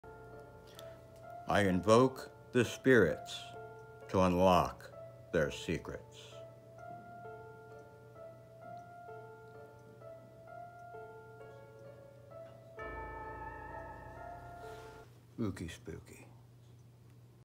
Ron Fitzgerald, Master Of The Dark Realm, Invokes The Spirits!
Actor and Gothic Illusionist, Ron Fitzgerald, Master of the Dark Realm performes his creepy ouija board magic in his strange and unusual style of magic and illusion performance. Is Master Ron a vampire?! V""V

Thank's For Watching!  V""V